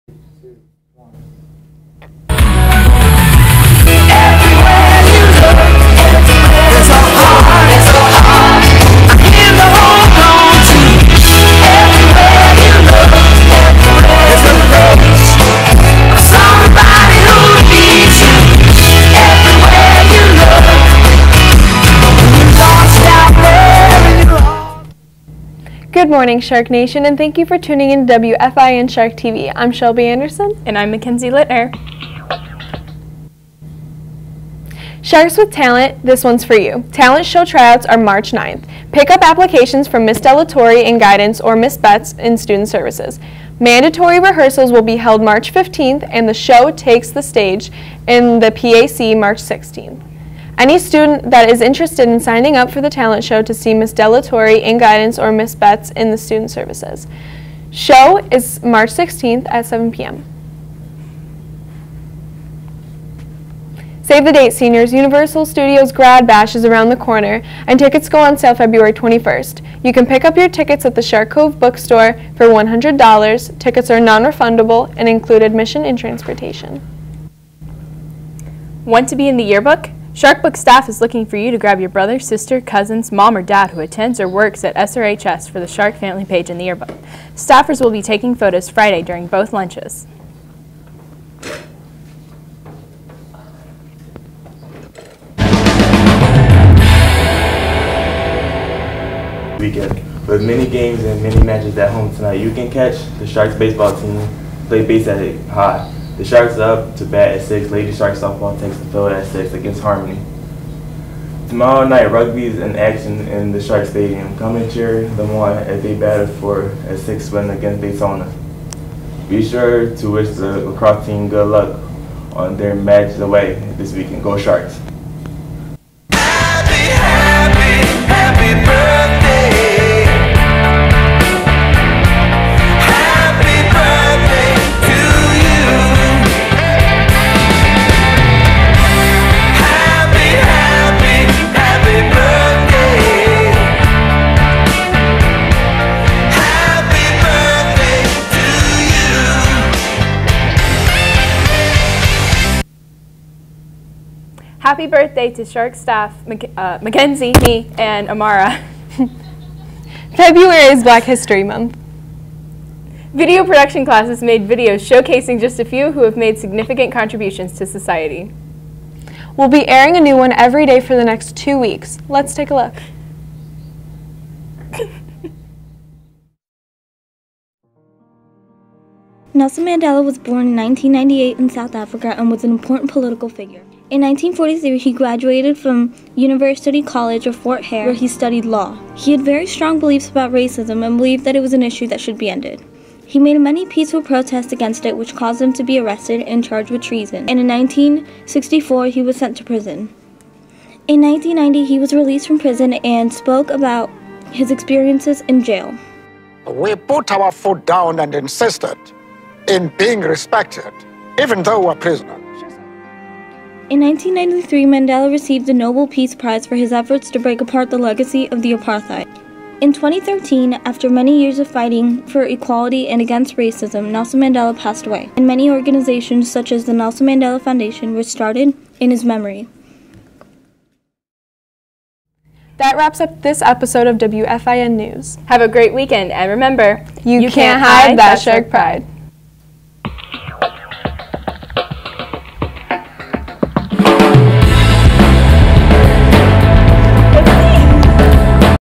WFIN-TV | 2-24-17 | Team Great Whites
Sebastian River High School's Student Newscast, featuring a mini doc on Nelson Mandela for Black History Month.